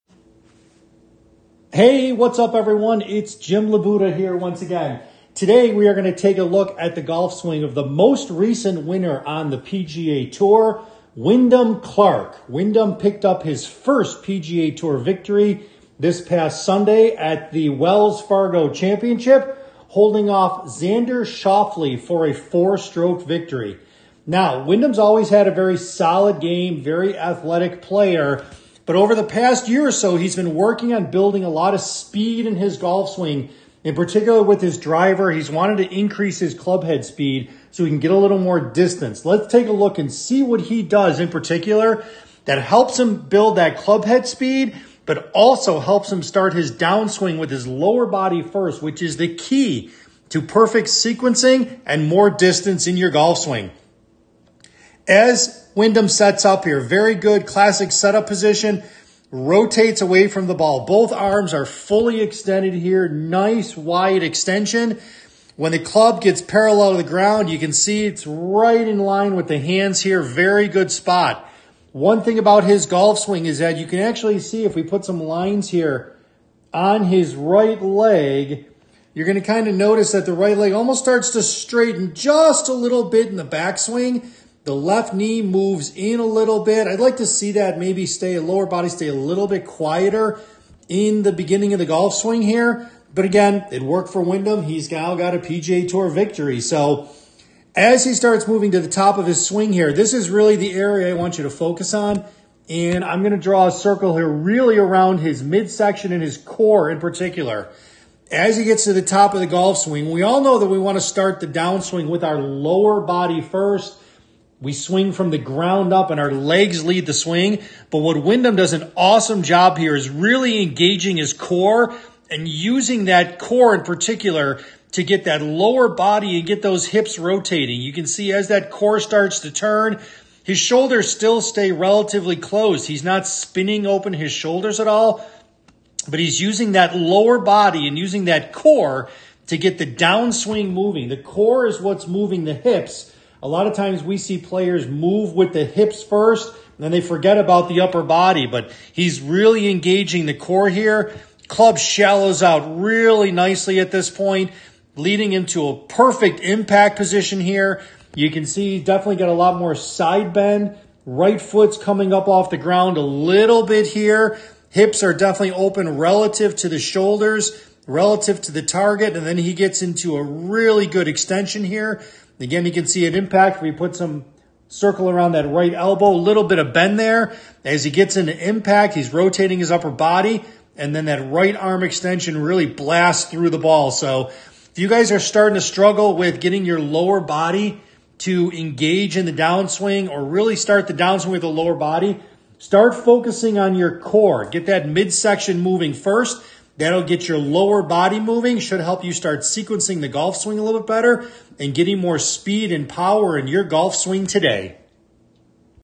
Wyndham Clark Golf Swing-Can One Core Move Really Improve Your Golf Swing?
Wyndham Clark picked up his 1st PGA Tour Victory this past Sunday at the Wells Fargo Championship.  He defeated Xander Schauffele by 4 shots to secure this first win.

Wyndham has always been a very athletic player but has recently been specifically working out to generate more speed in his golf swing.  He makes one KEY move in his golf swing with his core that can also help you generate more speed and power today!  Watch this video to the end to make sure you learn EXACTLY how to move your core for more speed and power in your golf swing!